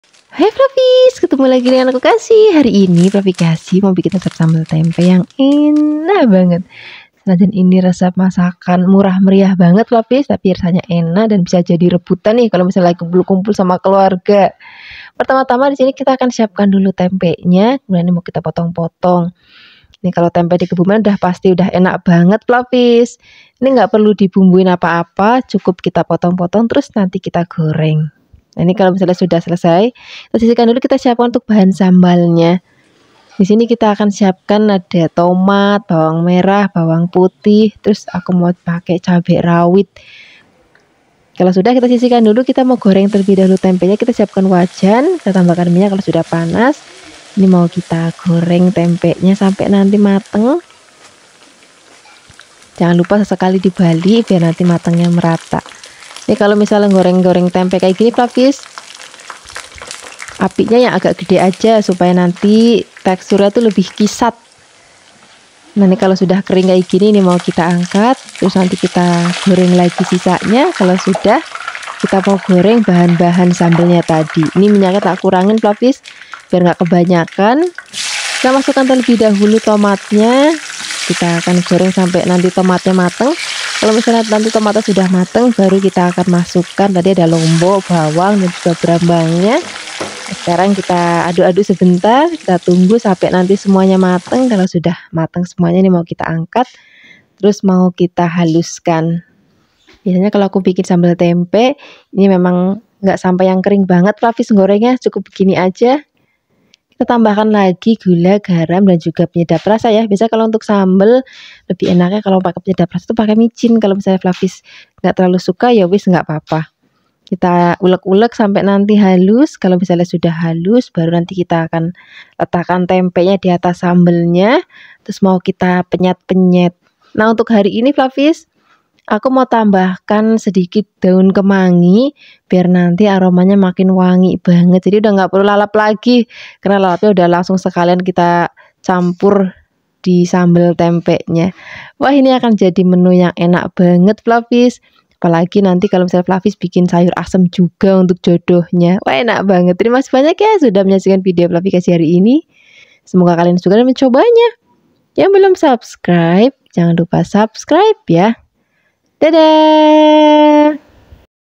nggak usah pake daging untuk menyajikan sarapan!kenapa aku baru tau
BAHAN:
-1 Tempe
-kemangi secukupnya
-2  tomat
-segenggam cabai
-2 bawang putih
-6 bawang merah
-garam secukupnya